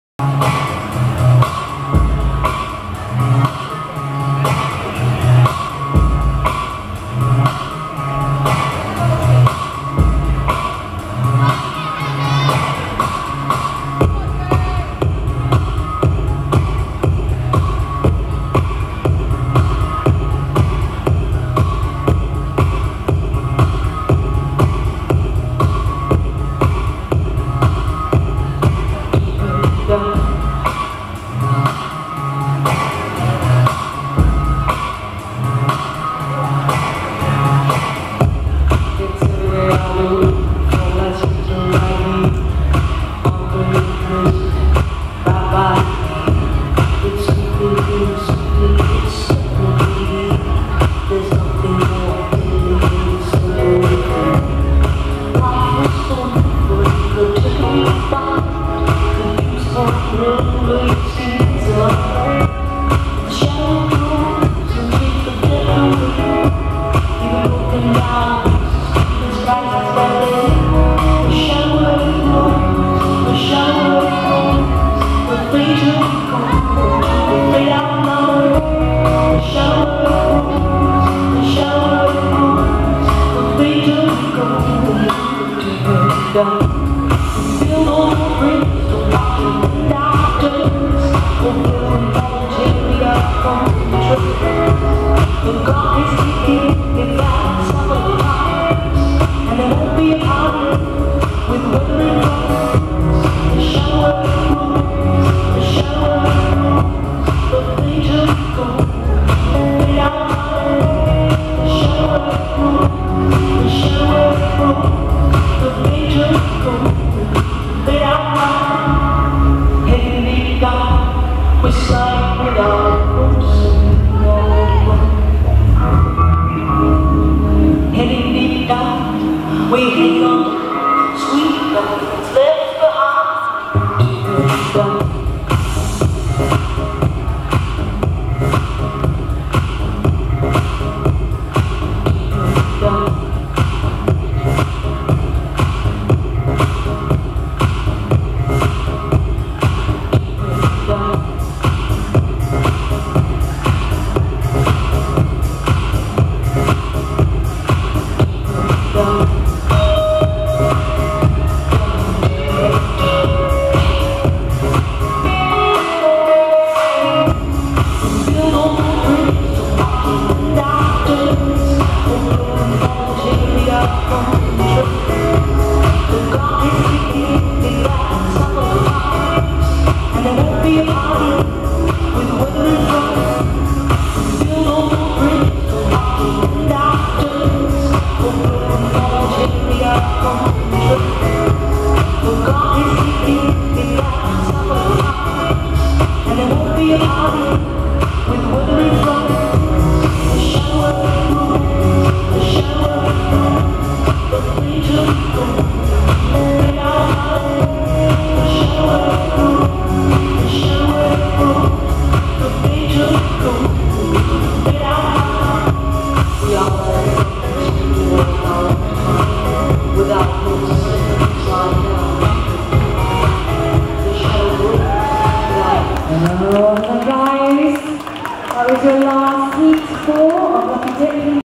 Welsh Champs 2018 - Lucky Dip Heat 4c
Lucky Dip Heat 4c at the Ceroc Welsh Champs 2018, recorded live at Barry Memorial Hall on 29th September 2018.